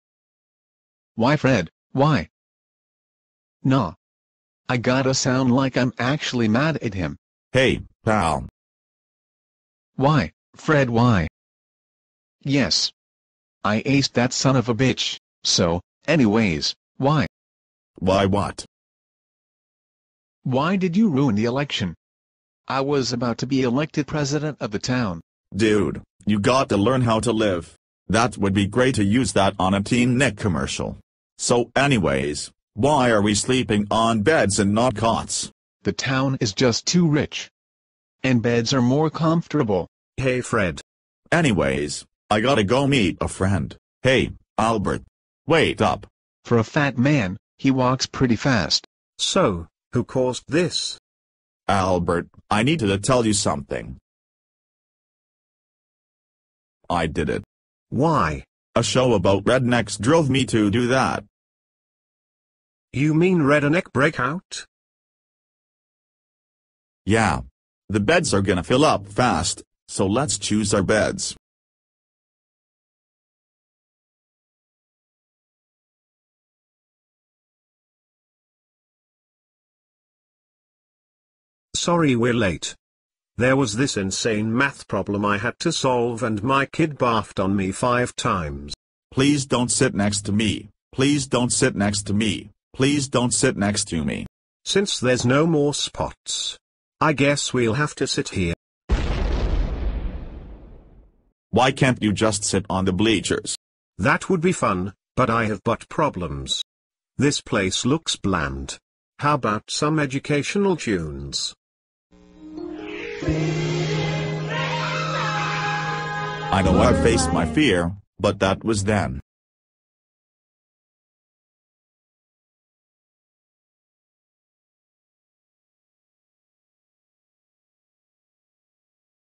Funny Fred and the Evacuation part 2
FunnyFredOfficialSite.piczo.com
YEAH! Nerdy Ned can Levitate, you didn't know that? and he can levitate his kid, too! You didn't know that either?In my opinion, the Explosion joke was better in Funny Fred and the Cabin Fever